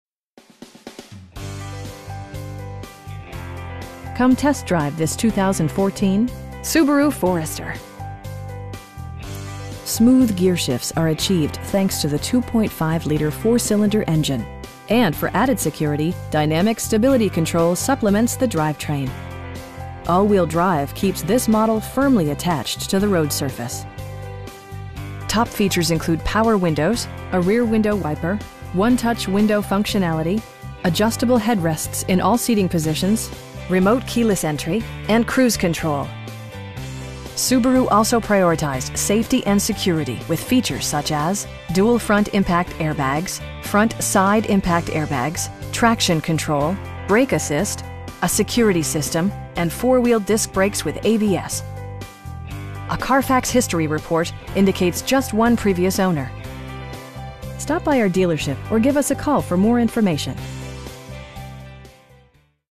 2014 Subaru Forester 2.5i in Stamford, CT 06902
Subaru Stamford
198 Baxter Ave

Come test drive this 2014 Subaru Forester. Smooth gearshifts are achieved thanks to the 2.5 liter 4 cylinder engine, and for added security, dynamic Stability Control supplements the drivetrain. All wheel drive keeps this model firmly attached to the road surface. Top features include power windows, a rear window wiper, 1-touch window functionality, adjustable headrests in all seating positions, front bucket seats, tilt and telescoping steering wheel, remote keyless entry, and cruise control. Subaru also prioritized safety and security with features such as: dual front impact airbags, front side impact airbags, traction control, brake assist, a security system, and 4 wheel disc brakes with ABS. A Carfax history report indicates just one previous owner. We know that you have high expectations, and we enjoy the challenge of meeting and exceeding them! Stop by our dealership or give us a call for more information.